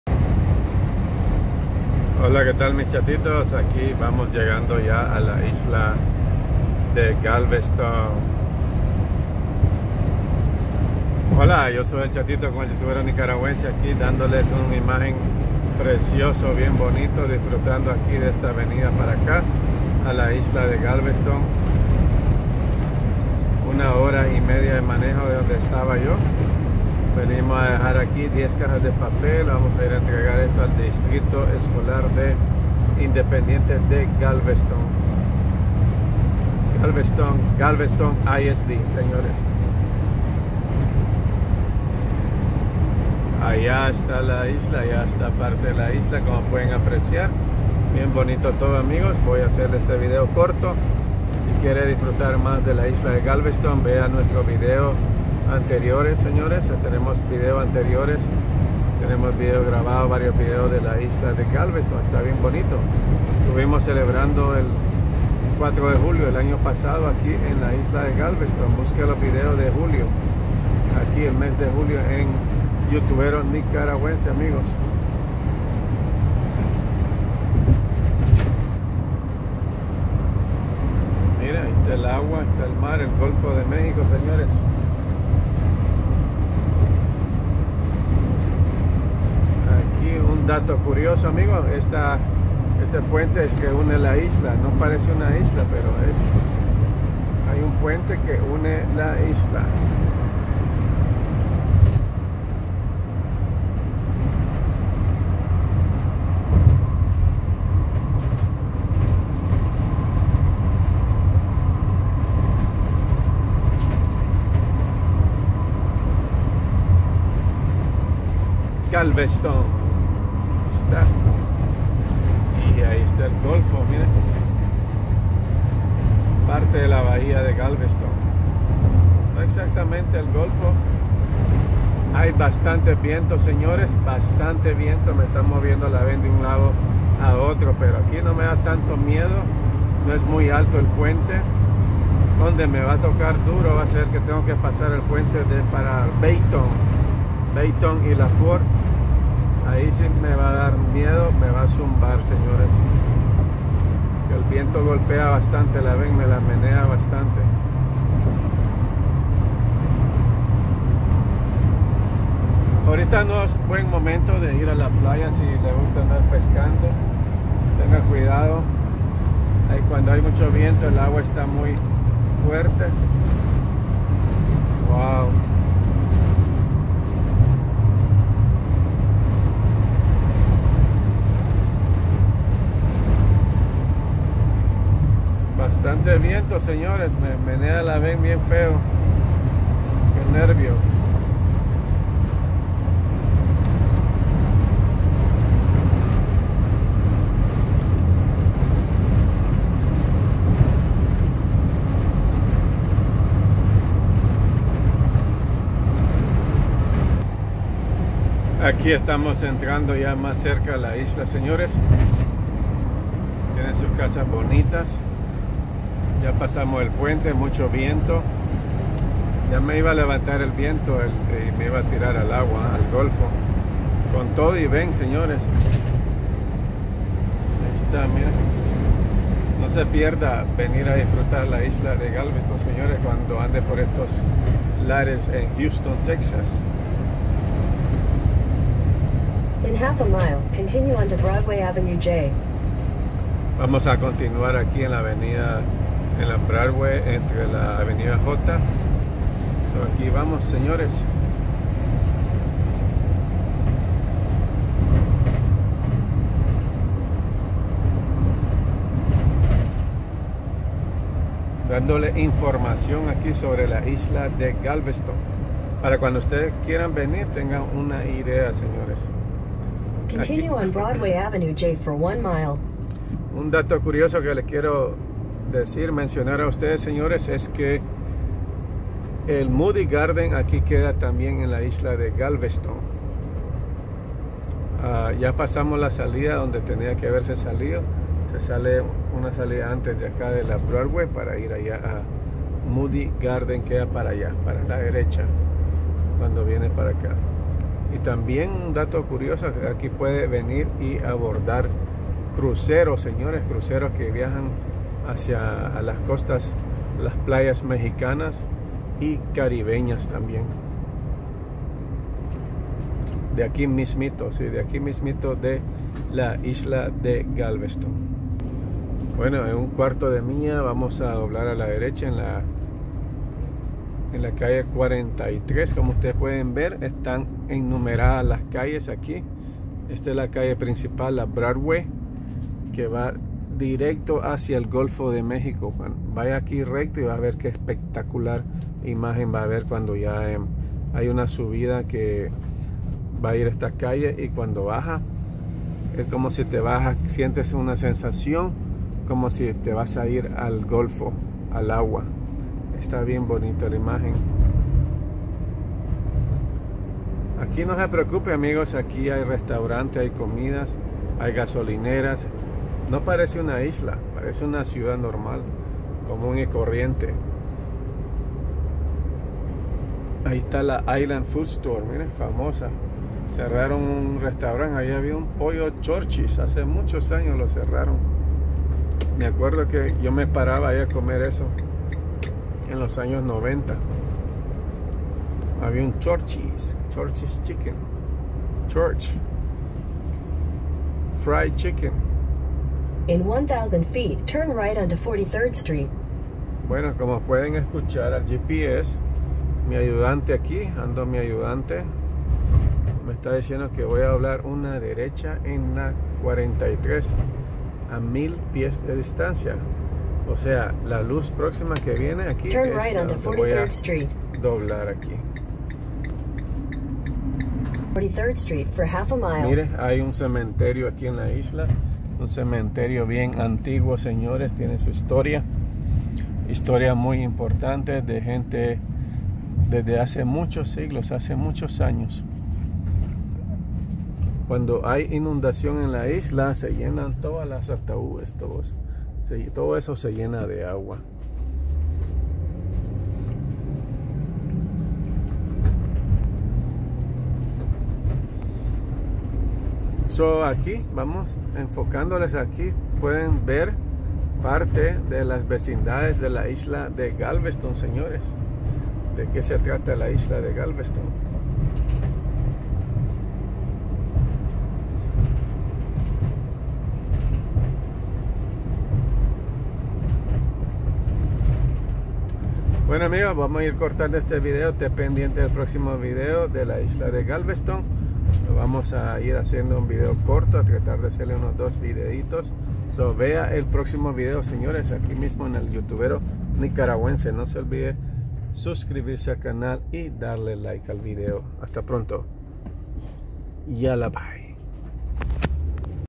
ISLA DE GALVESTON TEXAS# TURISMO
Conociendo la Isla de Galveston Texas